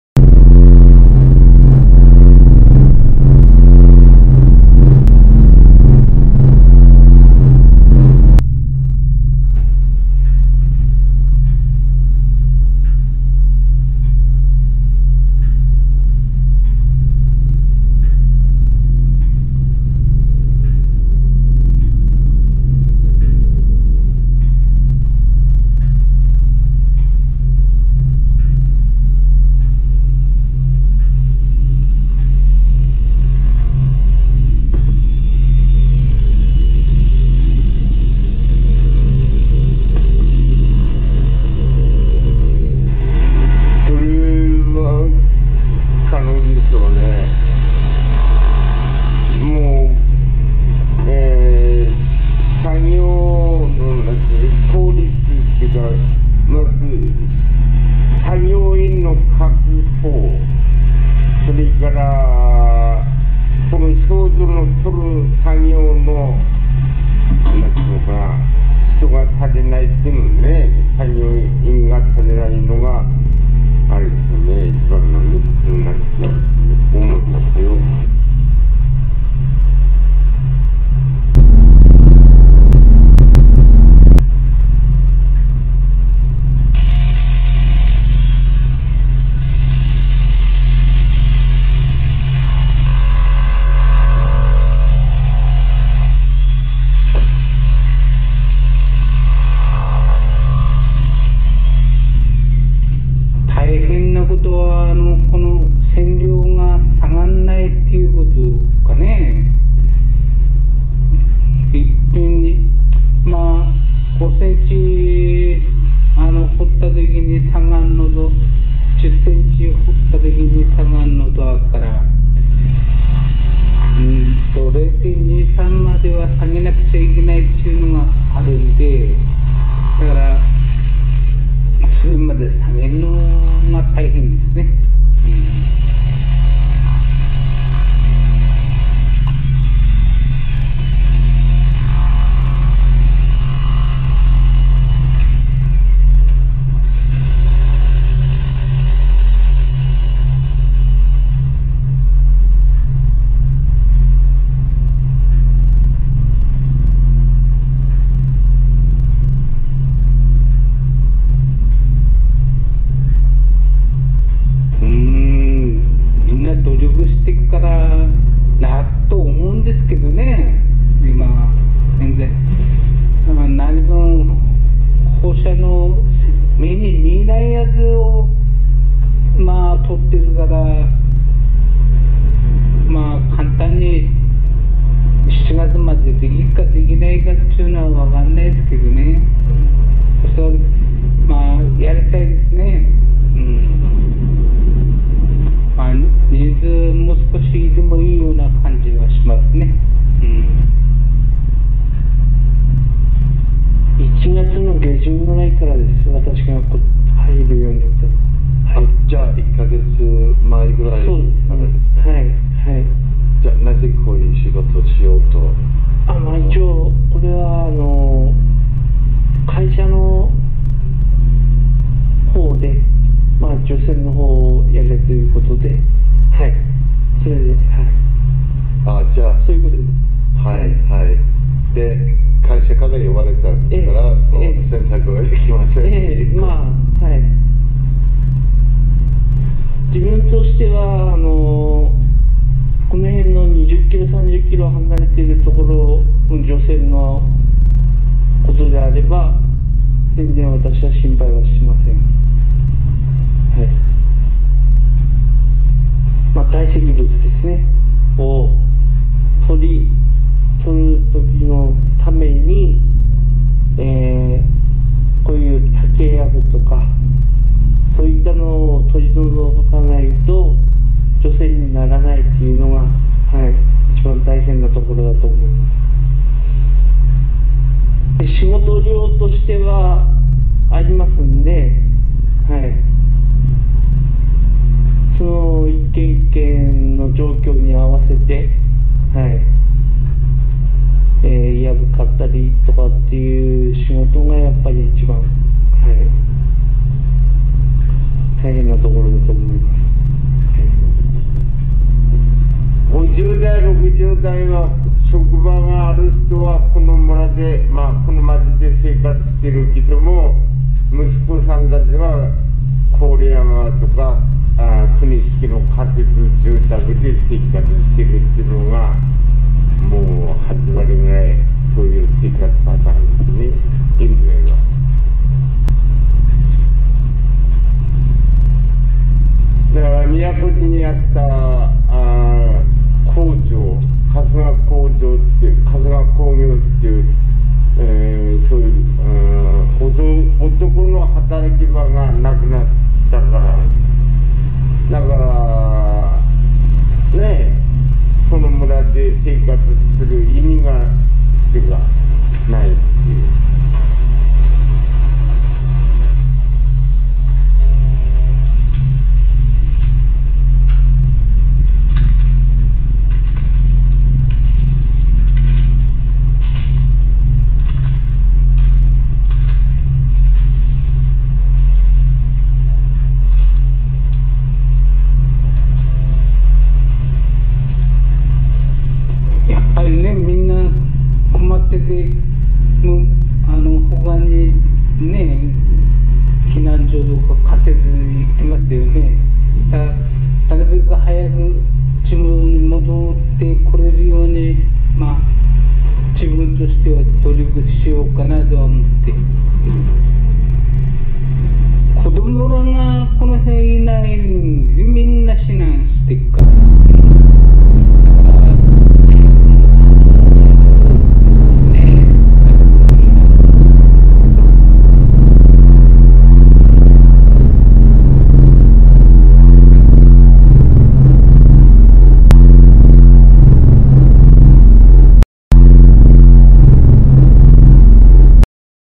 FUKUSHIMA decontamination
the reality of decontamination efforts while meltdowns continue . japan march 2013